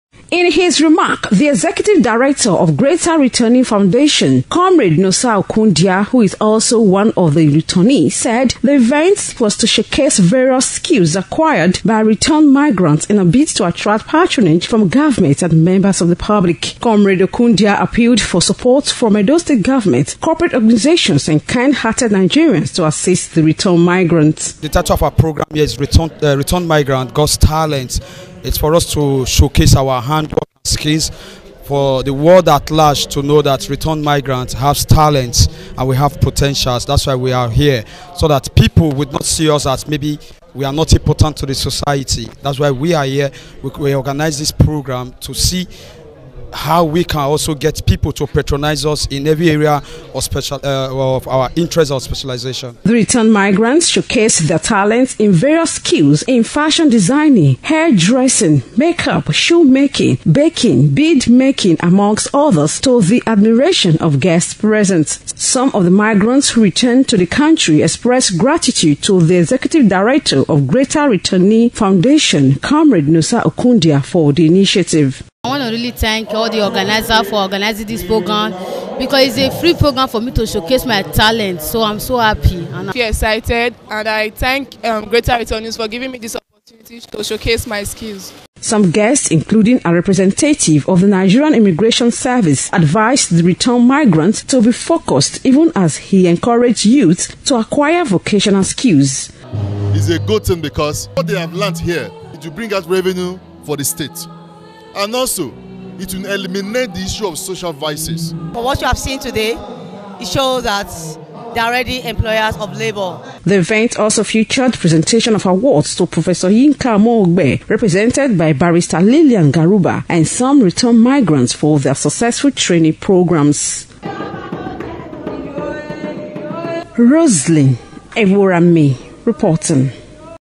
GRF holds maiden edition of Returned Migrants Got Talent 2020
Greater Returnee Foundation today held its maiden edition of Returned Migrants Got Talent 2020 amid funfair.

Roseline Ebhoramen reports that, the event held at Imaguero college located at sapele Road, Benin City also featured presentation of awards to some guests and returnee migrants.